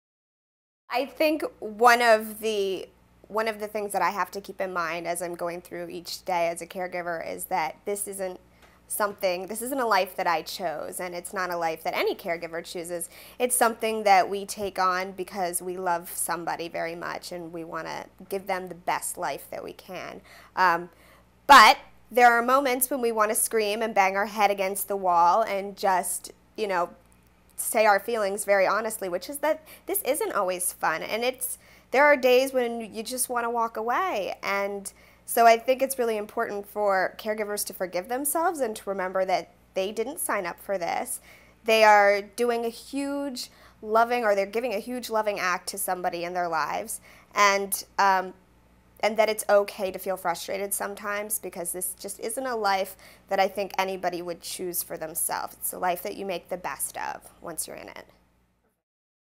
Caregiver Confessions: I Didn't Sign Up for This Gig
Caregiver Abby Maslin talks about the fact that life with a TBI  isn't a life anyone would choose, rather one you have to make the best of.